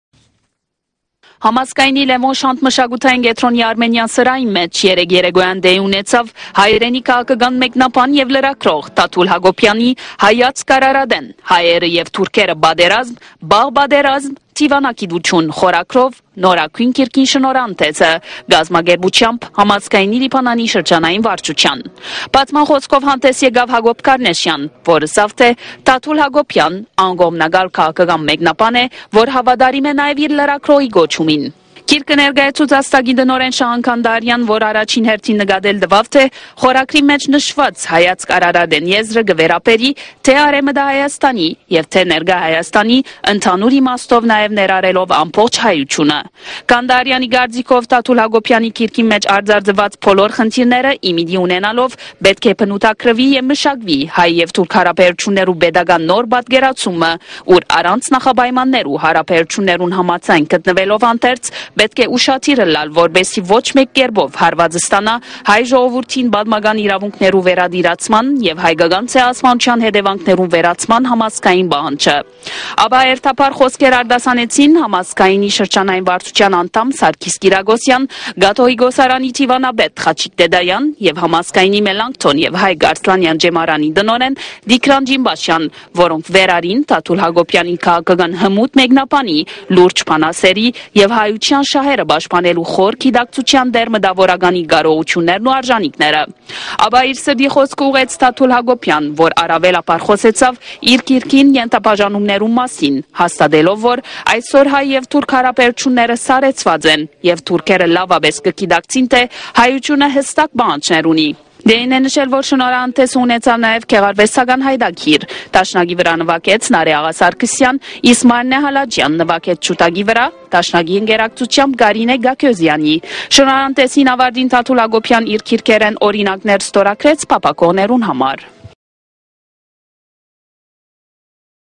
Հայեացք Արարատէն. Հայերը և Թուրքերը գիրքի շնորհանդէսը Պէյրութի մէջ` 21 Մարտի, 2012թ
Մարտի 21, 2012թ